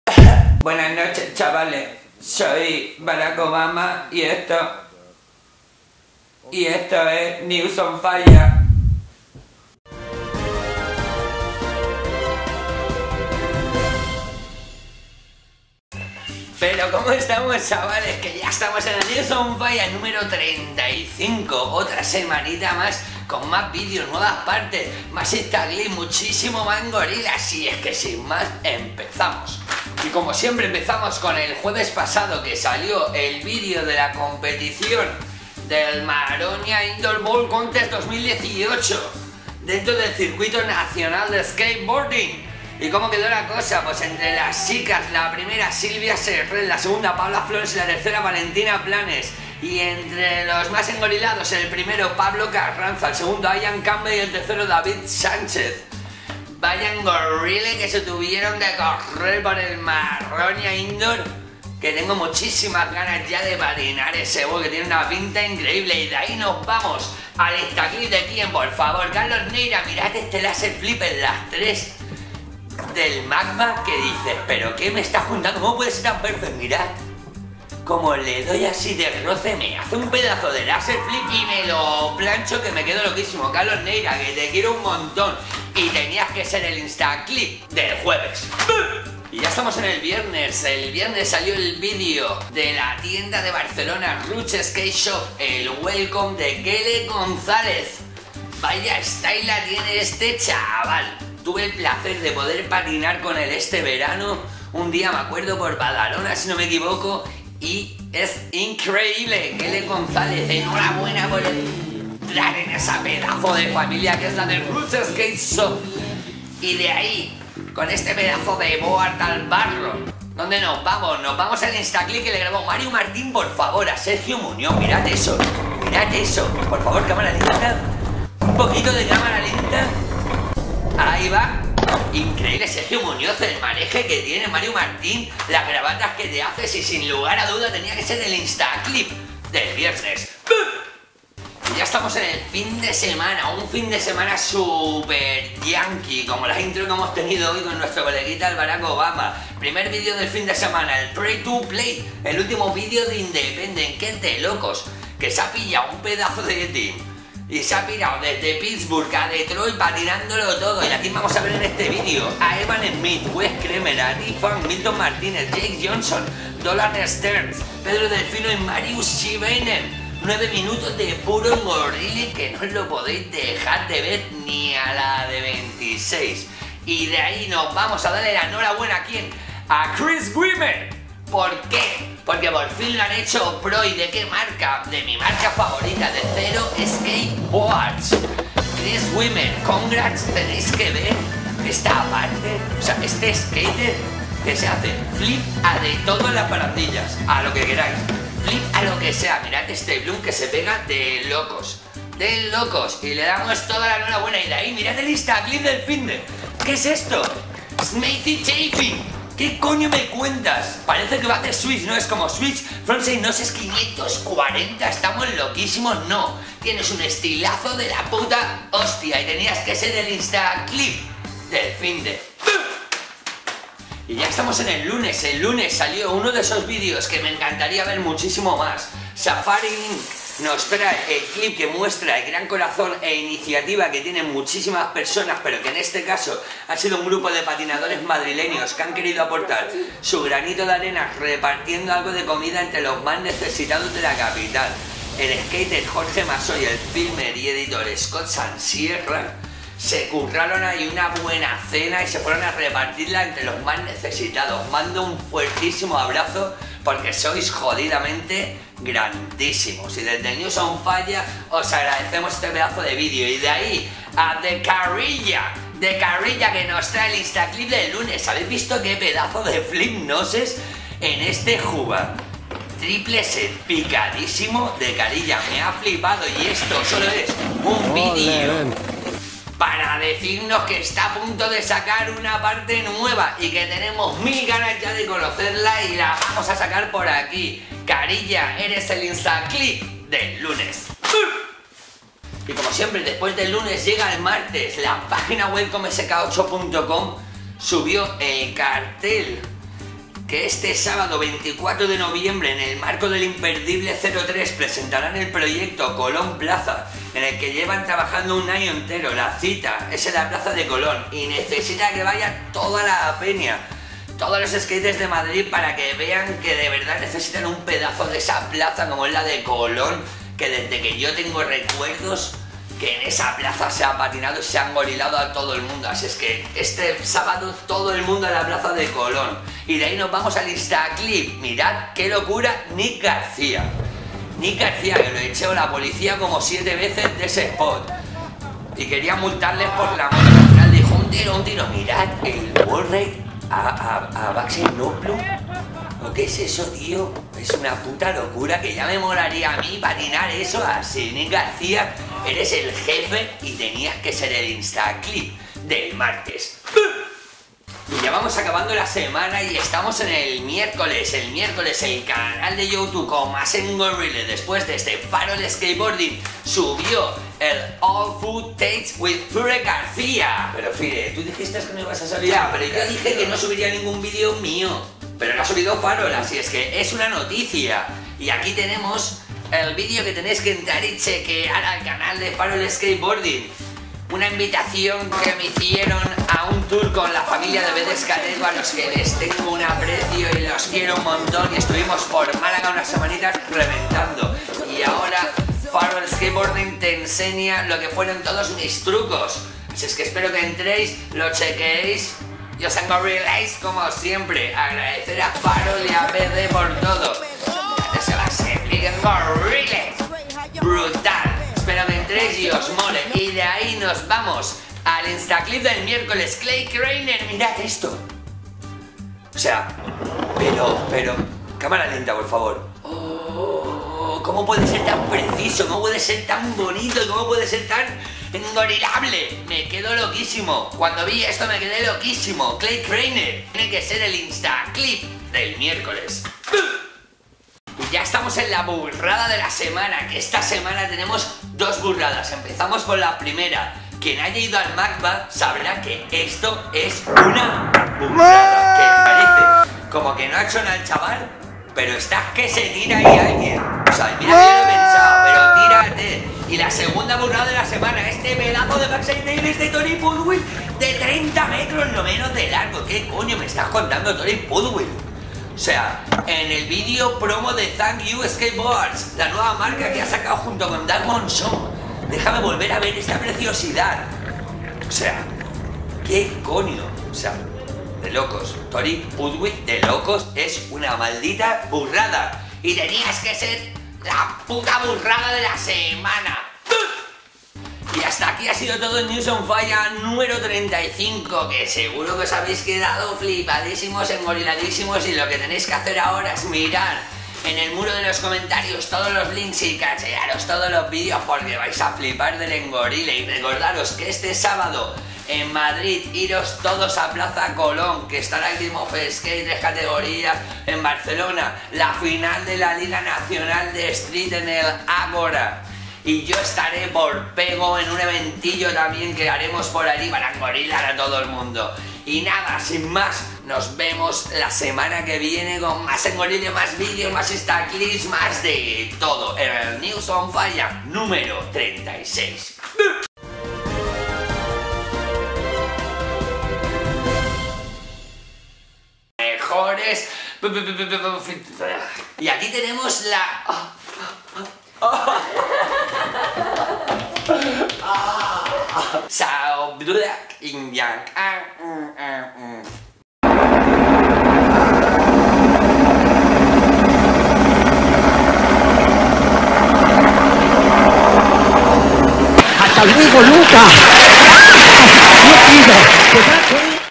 NEWS ON FHIRE 35! CARLOS NEIRA, SERGIO MUÑOZ, CHRIS WIMER, NICK GARCIA, SKATEBOARDING
-NEWS ON FHIRE Nº 35

-Noticia  Jueves  X  C.N.S Maroña Indoor Bowl Contest 2018

Circuito Nacional CLASIFICACIONES FEMENINAS
1 Silvia Serret  2 Paola Flores  3 Valentina Planes

-CLASIFICACIONES MASCULINAS
1  Pablo Carranza 2  Ian Campbell 3  David Sánchez

Insta  Jueves  X Carlos Neira @aire.neira

Noticia  Viernes X  Gelek Gonzalez ROOTS Skateshop

Insta  Viernes X  Sergio Muñoz @sergiomunozskt

Noticias Finde X  Independent "Gotta Pray to Play" Video

Noticias Finde  X  Zero Skateboards Chris Wimer "In Your Head" Part.

Insta Finde X  SHMATTY CHAFFIN  @shmatty

-Noticia Lunes X  Safari Inc. nos trae este clip que nos muestra el gran corazón e iniciativa que tienen muchísimas personas, pero en este caso han sido un grupo de patinadores madrileños que han querido aportar su grano de arena repartiendo algo de comida entre los más necesitados de la capital. Skater Jorge Masó, filmer y editor Scott Sansierra

-Insta  Lunes  X El Kariya  @thekariya

-Noticia Martes X  WELCOMESK8.COM Este sábado 24 de noviembre, en el marco del Imperdible_03 presentaran el proyecto COLÓN PLAZA en el que llevan trabajando un año, la cita es en la Plaza de Colón y necesitan la participación de todos los skaters madrileños para que vean que de verdad necesitan un pedazo de la plaza de Colon.

El evento consistirá en un Game of S.K.A.T.E y un best Trick en un modulo sorpresa!! Habrán 3 categorías en los dos campeonatos -16 +16 y Chicas. Y mogollón de premios en material para todas las categorías.

-Insta Martes X  Nick Garcia @nick_garcia

-Noticia Miércoles X  Farol Skateboarding OLD FOOTAGE WITH FHIRE GARCIA

-Insta Miercoles X   Clay Kreiner @claykreiner

-La Burrada de la semana  X Felipe Suarez @goldieratt

Thank You Skateboards Torey Pudwill

Tag us to be featured

Y ya sabes nos vemos el próximo jueves con...

Más Noticias, más Vídeos, más Info y sobre todo más

ENGORILEEE!

-Saludos a todos los que disfrutéis de este trabajo y millones de GRACIAS. :)

-Love & Patín.